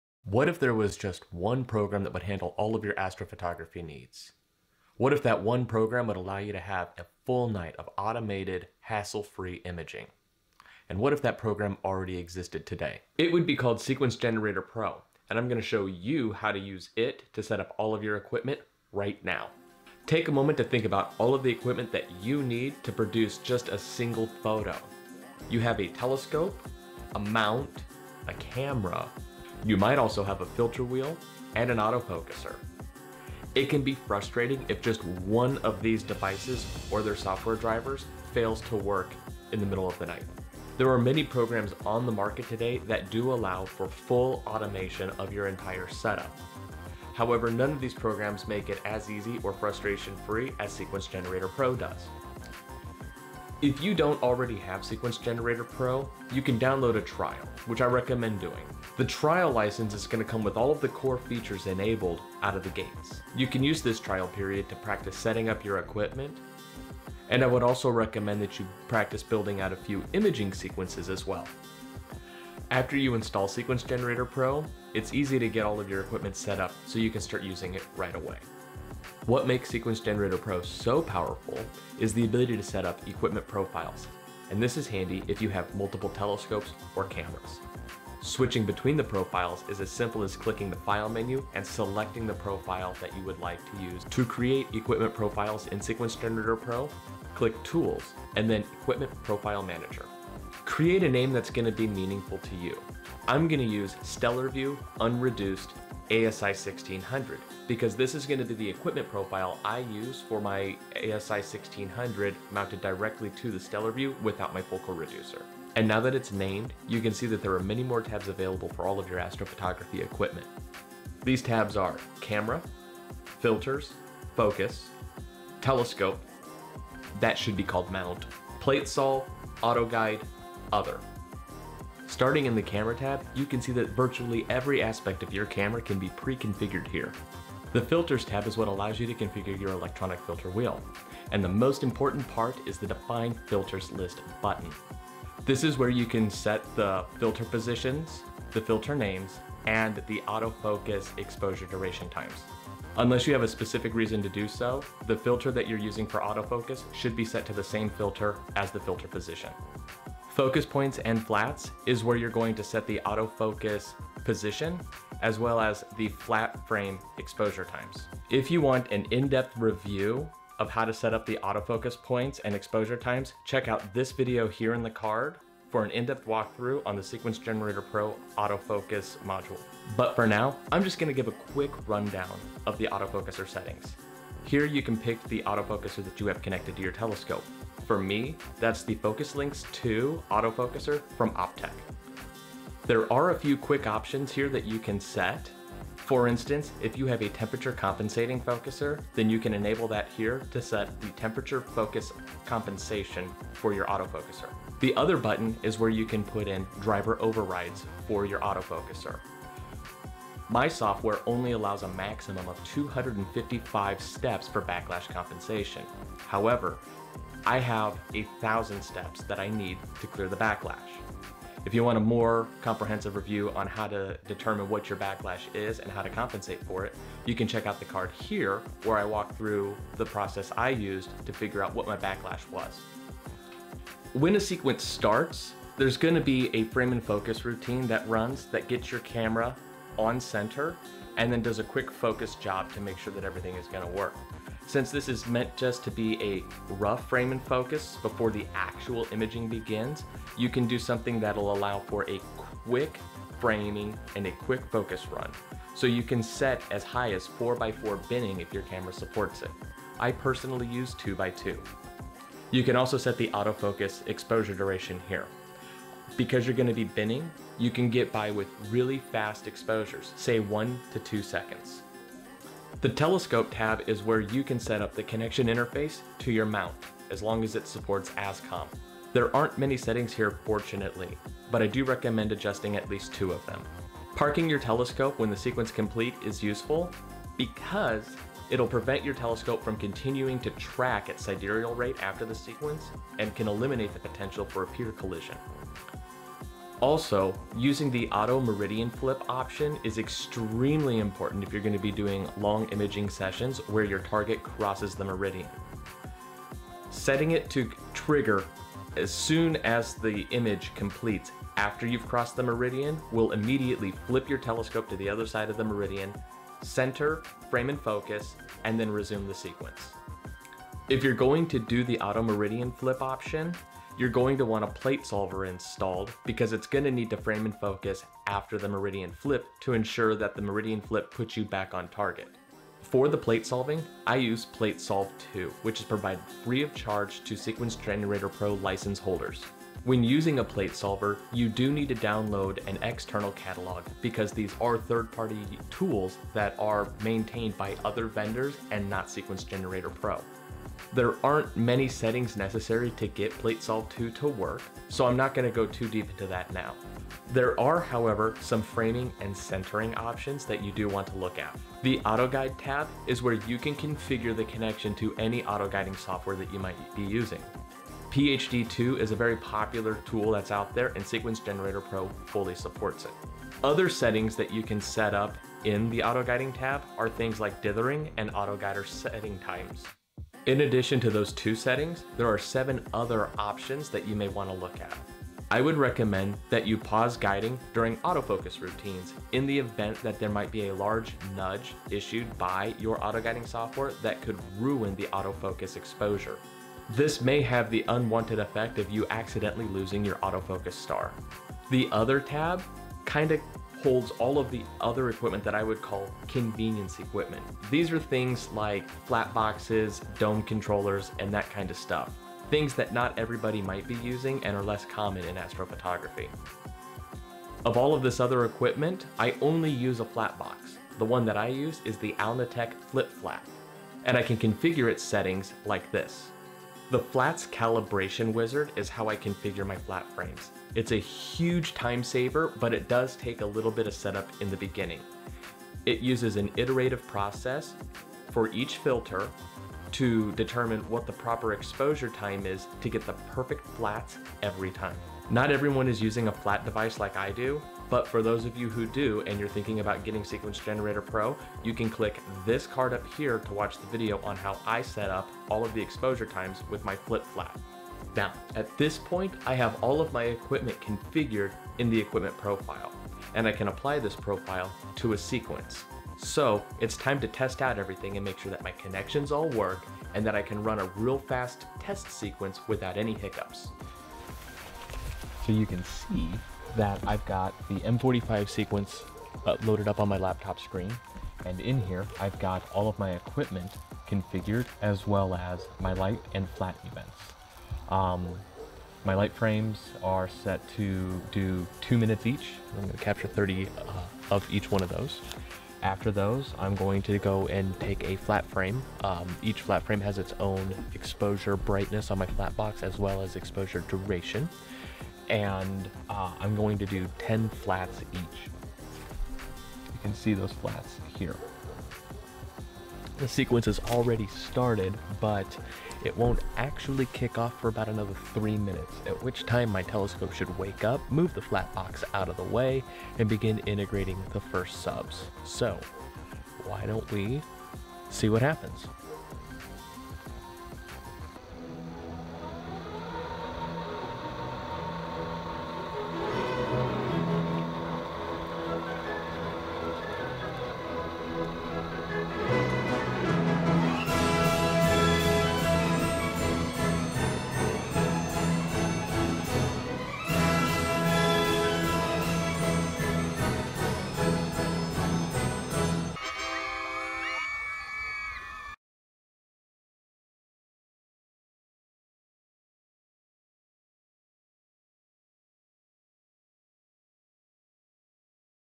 Sequence Generator Pro Setup Your Equipment | Automated Astrophotography
If you need a quick rundown of how you can use Sequence Generator Pro to setup all of your astrophotography equipment for a night of automated astrophotography then you're in the right place!

Sequence Generator Pro is a powerful automation tool that can connect to all of your astronomy gear and let you create complicated sequences for imaging.  You can even image over the course of multiple nights, hitting multiple targets per night (more on that in a future episode).

Using SG Pro can be a bit of a learning curve but it really is easier than you think.  This video gives you a quick walk through of my equipment, how it is configured for me, and where certain options you'll want to investigate are located.

If I've left out anything, or you need better explanations for setting up something in Sequence Generator Pro, drop me a comment below and I'll get the idea in to the works!